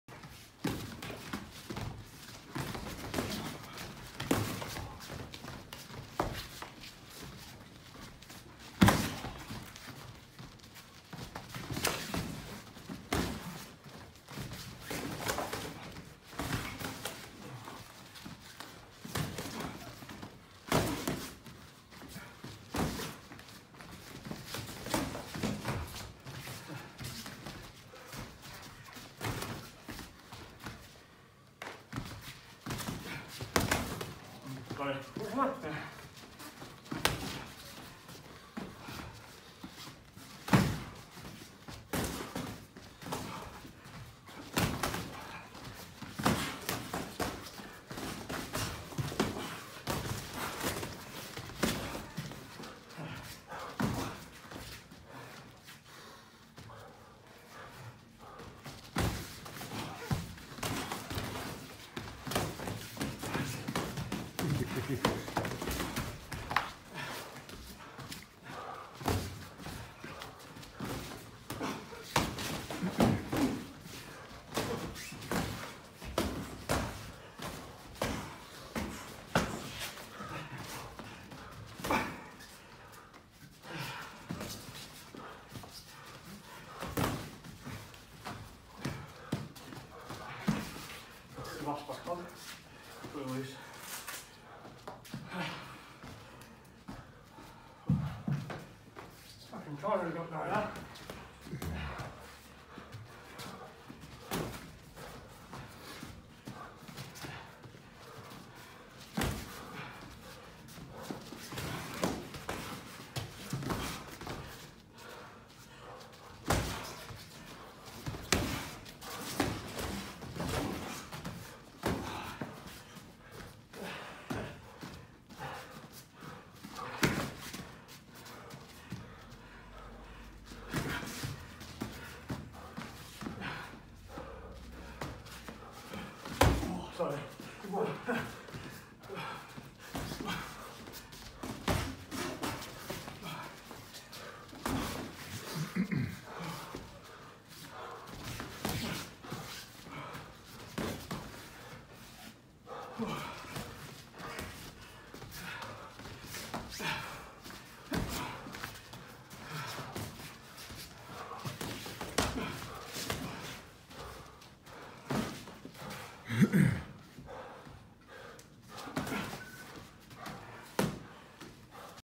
Boxing training
Just a bit of fun adapting styles to help out another martial artist with his white collar boxing debut.